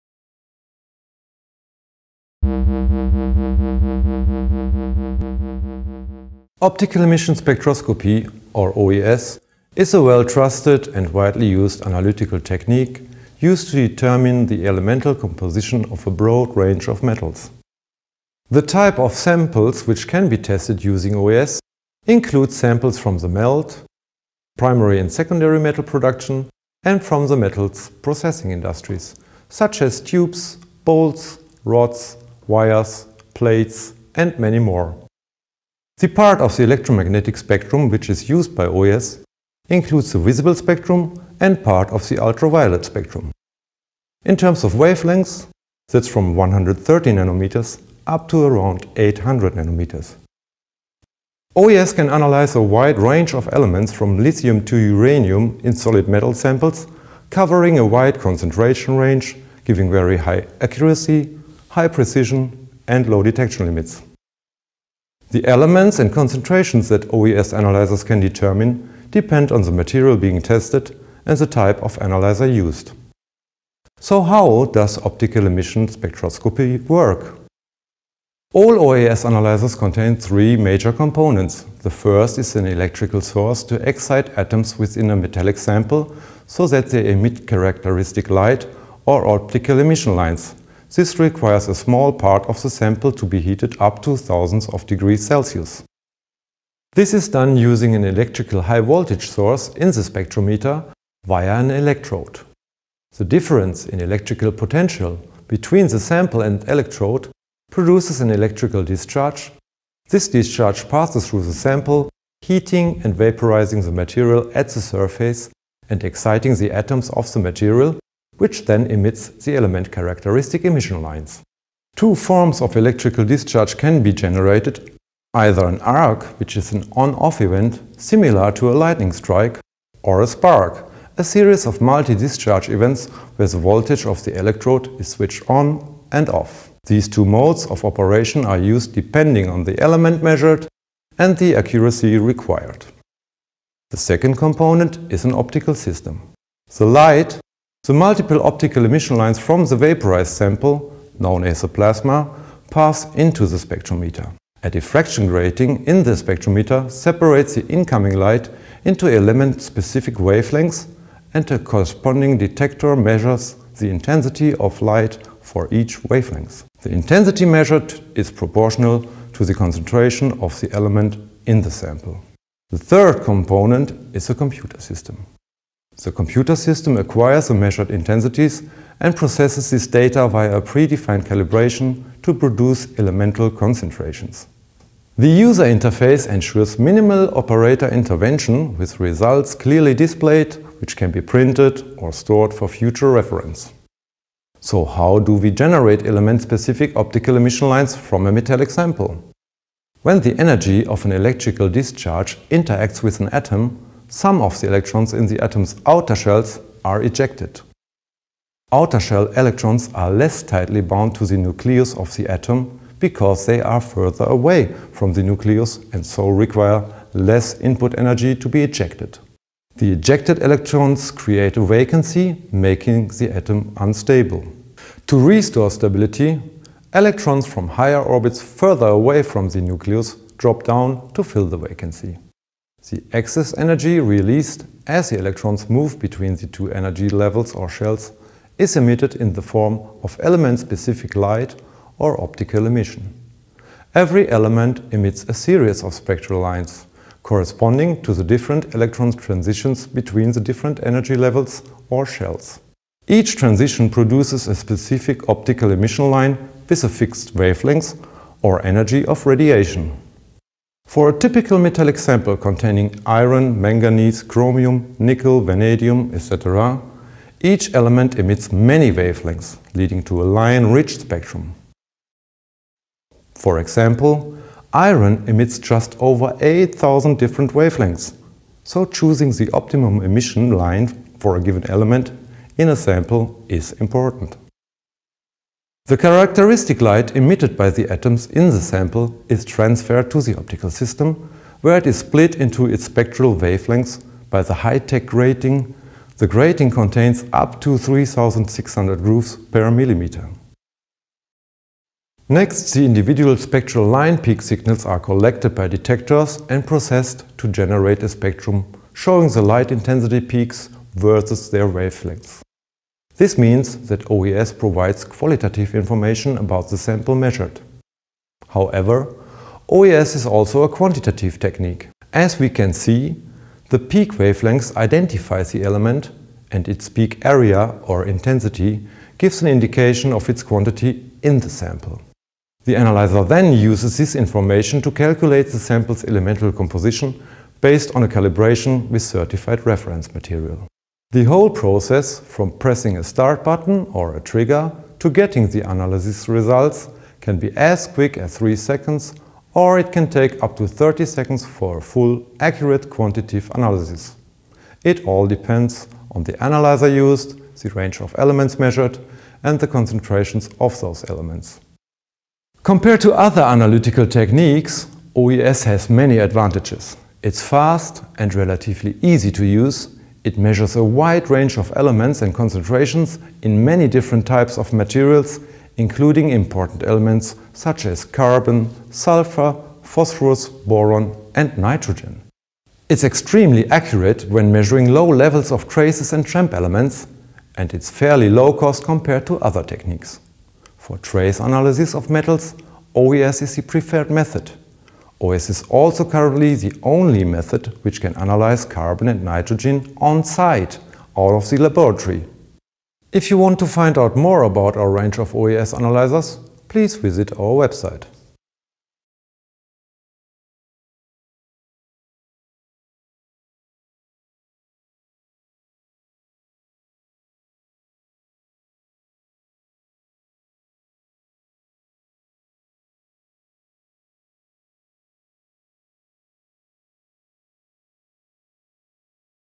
Что такое оптико-эмиссионная спектроскопия? Объяснение принципов ОЭС
Менеджер линейки ОЭС компании Hitachi High-Tech Analytical Science Willy Sanders рассказывает о принципах работы ОЭС.
Узнайте больше о наших OЭС инструментах и как они могут помочь Вашему бизнесу на